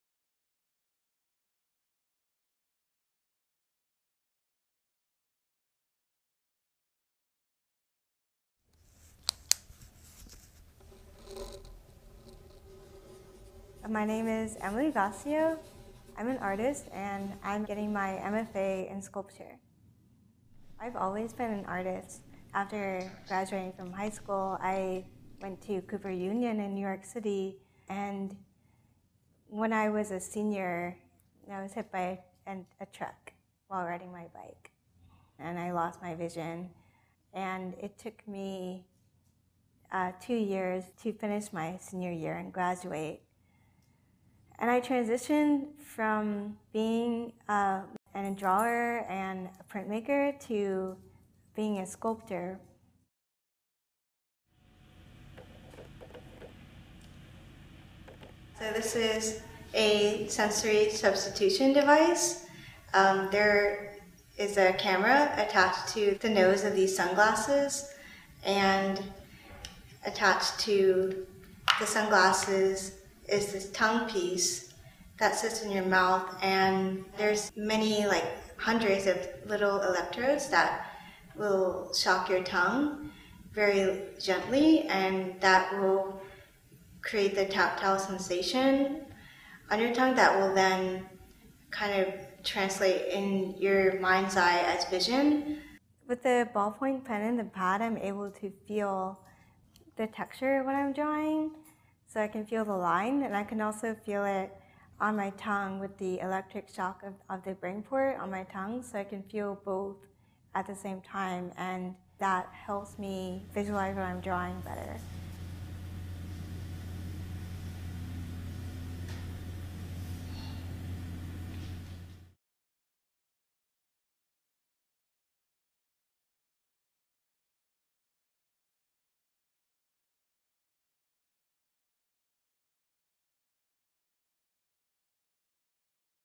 Access+Ability: BrainPort, Featuring Emilie Gossiaux
BrainPort is based on the proposition that one sees with the brain, not with the eyes. Embedded in the headset is a camera, a surrogate “eye,” that translates the shapes of things in the physical world into vibrations that can be felt through a device placed on the user’s tongue. With practice, one can learn to interpret the vibrating patterns and actually start to see.

On view at Cooper Hewitt, Smithsonian Design Museum, as part of Access+Ability, from through Sept. 3, 2018.